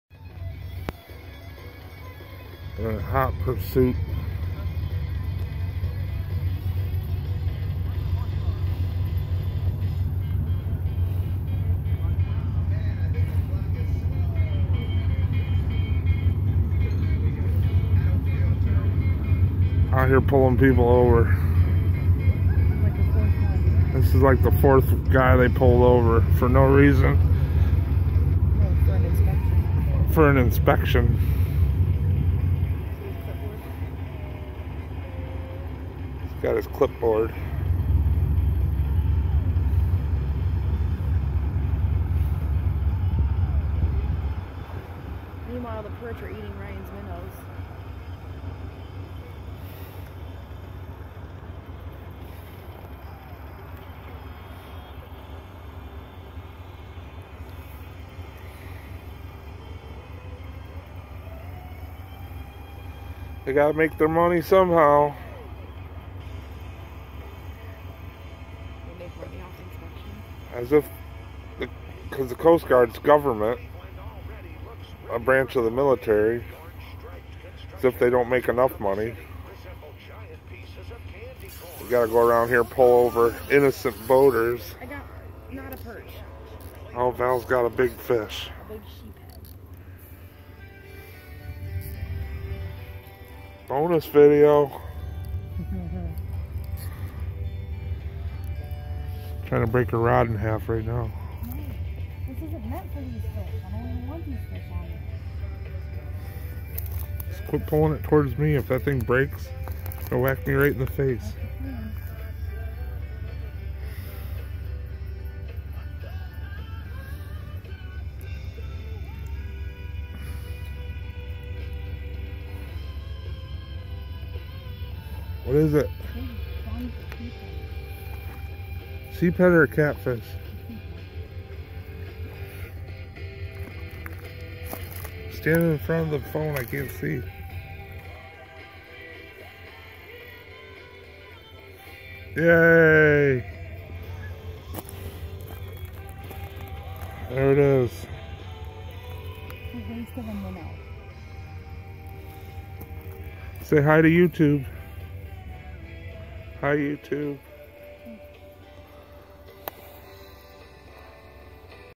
Valerie catches a sheepshead (freshwater drum) and boaters get inspected by the coast guard.
West Harbor channel, yellow perch fishing.